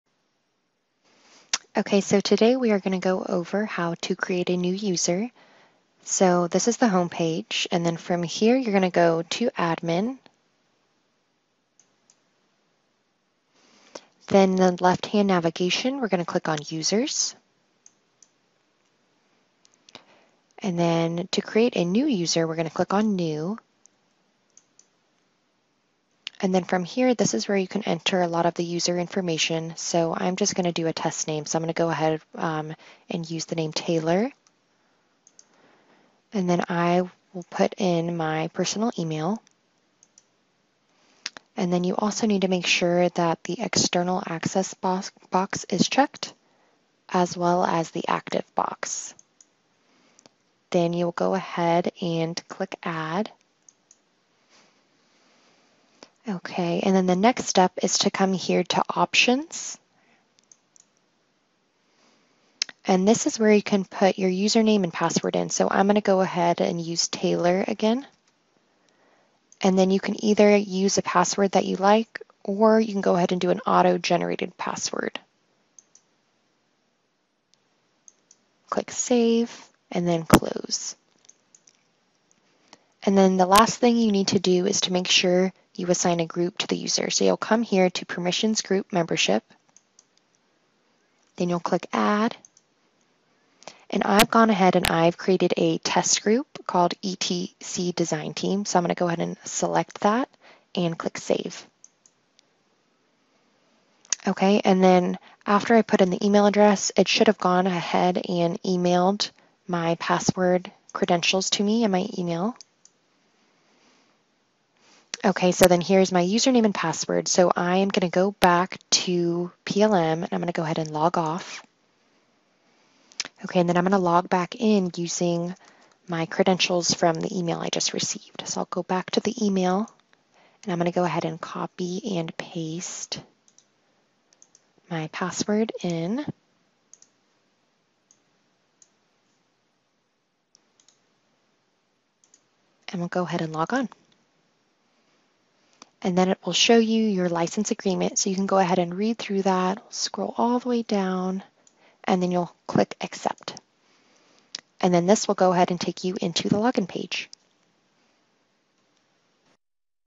YuniquePLM: Creating a New User
Learn how to create a new user in YuniquePLM in this video outlining the steps. Questions?